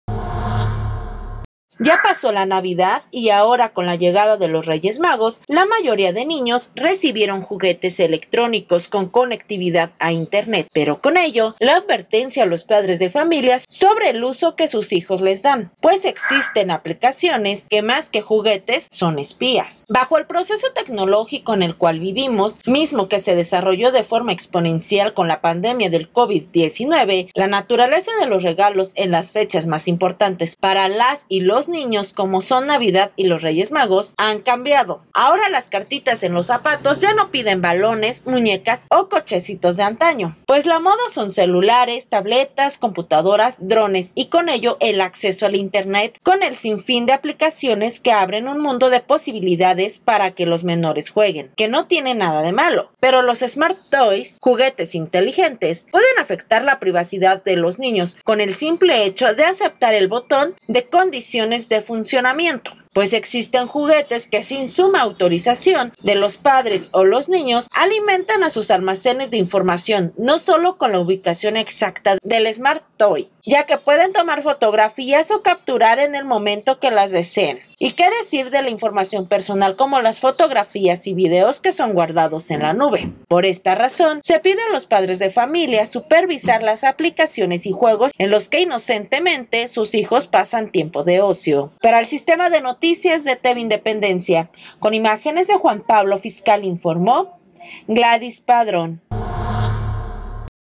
Cuidado con los juguetes tecnológicos
Ya pasó la Navidad y ahora con la llegada de los Reyes Magos la mayoría de niños recibieron juguetes electrónicos con conectividad a internet, pero con ello, la advertencia a los padres de familia sobre el uso que sus hijos les dan, pues existen aplicaciones que más que juguetes son espías.